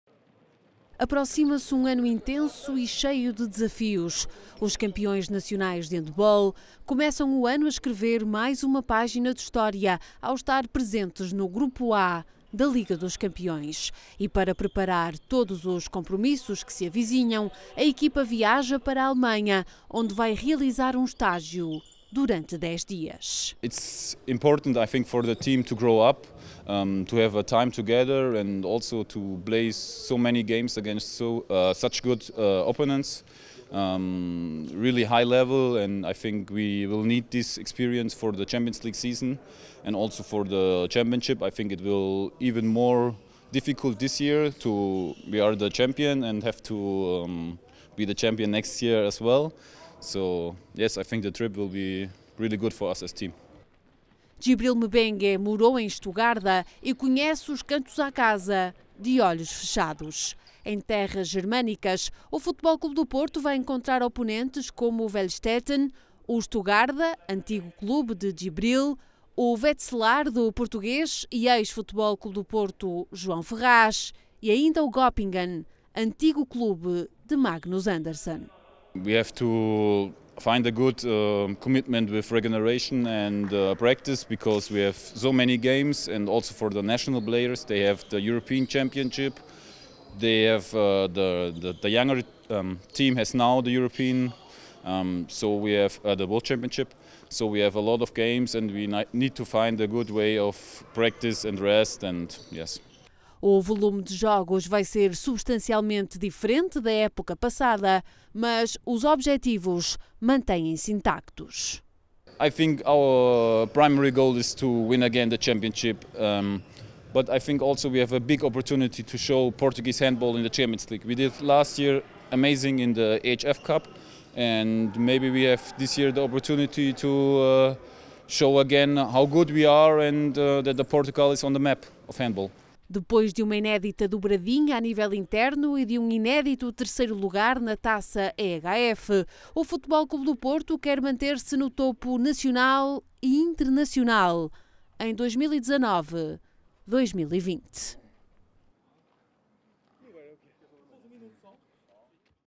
Djibril M’Bengue: "Vai ser importante para a equipa crescer"
O FC Porto viaja esta sexta-feira para Estugarda, na Alemanha, para um estágio de dez dias, nos quais vai defrontar adversários como o TVB 1898 Stuttgart, o HSG Wetzlar e o Frisch Auf Goppingen, bem como participar na Sparkassen Cup. Em declarações ao Porto Canal, Djibril M’Bengue fez a antevisão do estágio, que será “importante para a equipa crescer”, para que os Dragões estejam na máxima força para “enfrentar qualquer desafio”.